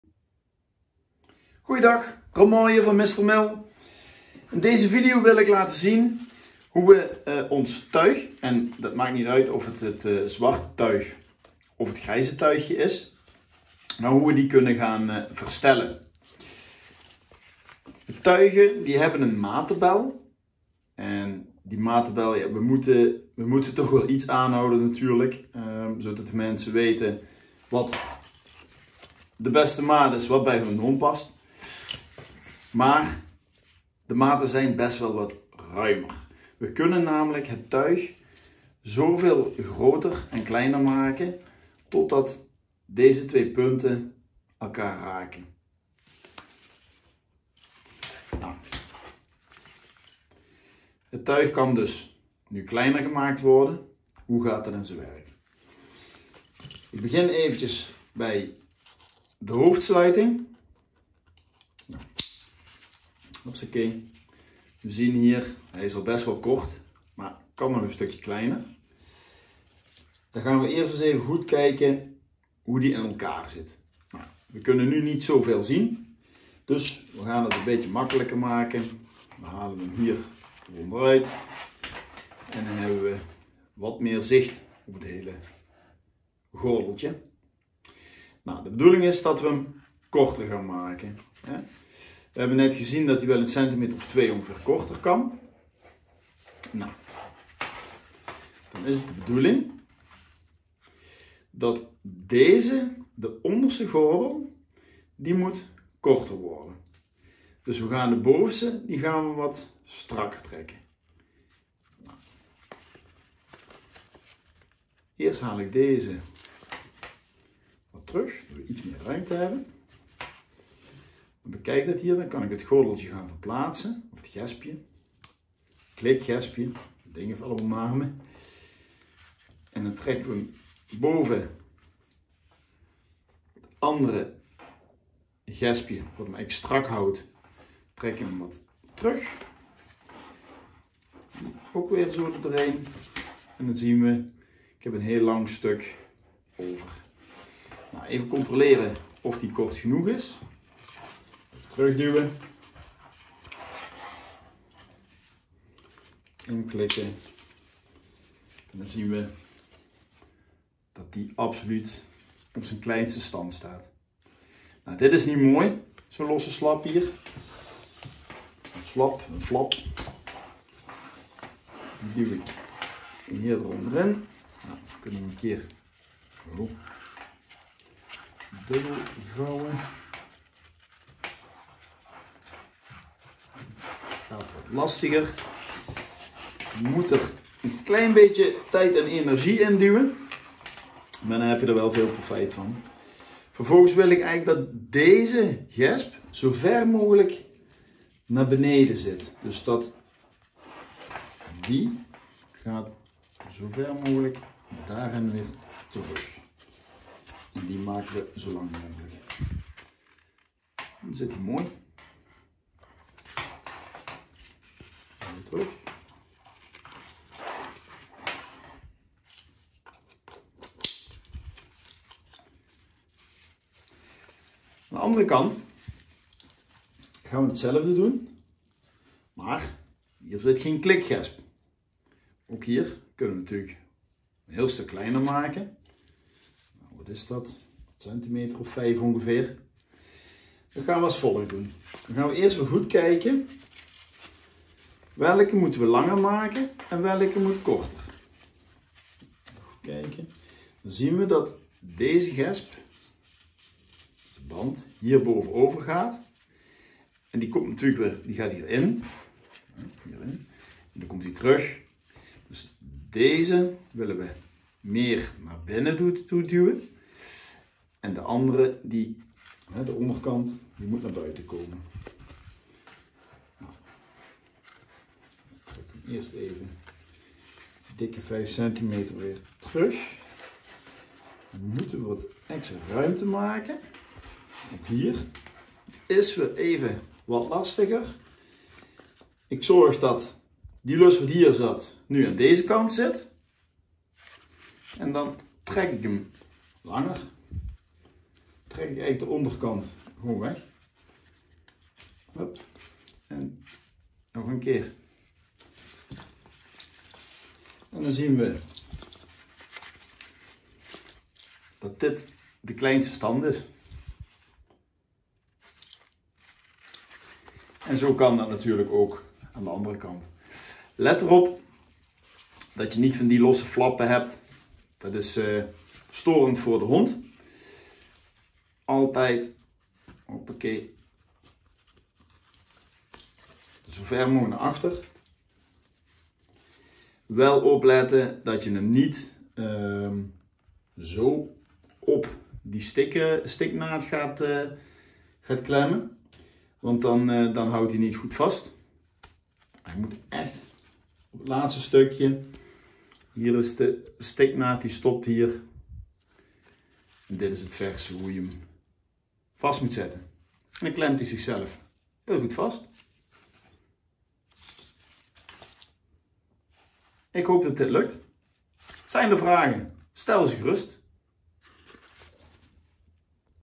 Mister Mill Y Tuig Hoe maak ik het tuig kleiner
Video Deel 1: Hoe maak ik de nekband kleiner van het Y Tuig / Anti Trek Tuig